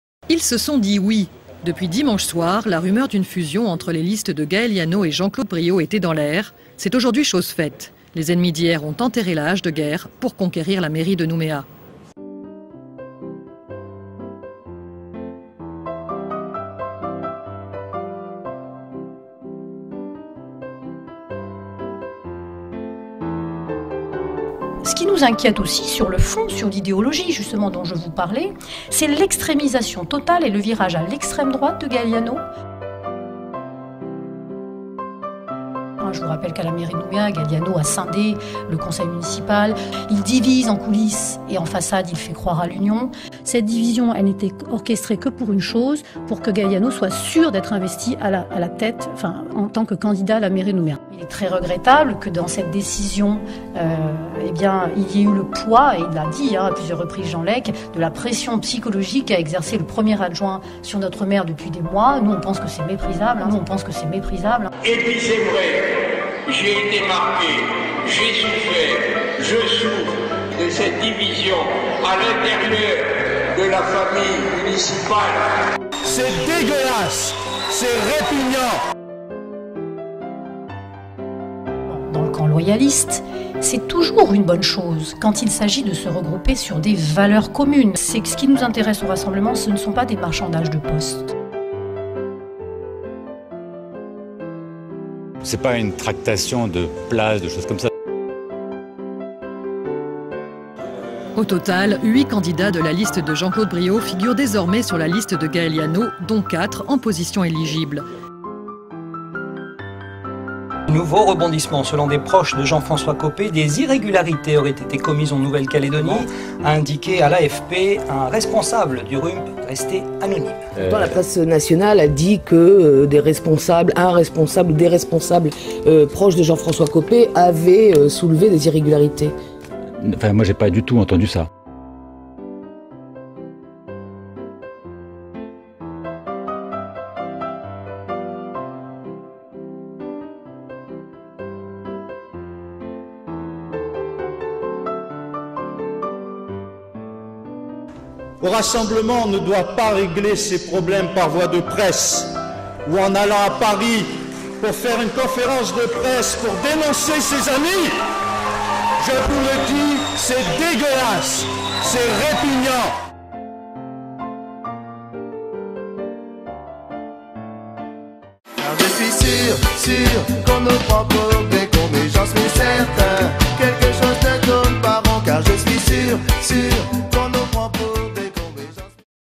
Épisode 5 : les dessous du mariage de Gaël et Gaby - Mars 2014
Le bon, la brute et le sacripant
Pour en savoir plus sur vos responsables politiques calédoniens, une petite vidéo vaut mieux qu'un long discours.